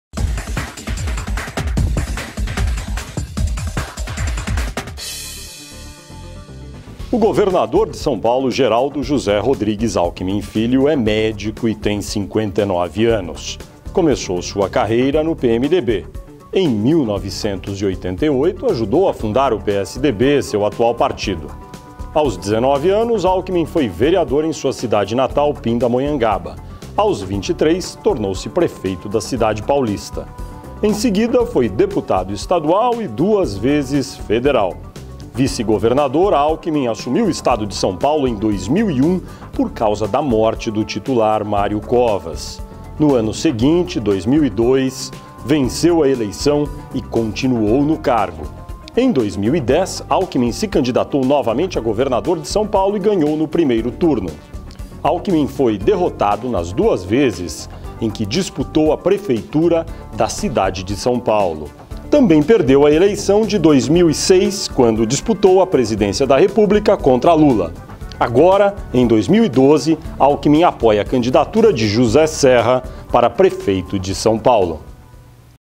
Quem é Geraldo Alckmin?
Governador de São Paulo, filiado ao PSDB, falou ao UOL e à Folha em 16 de agosto de 2012.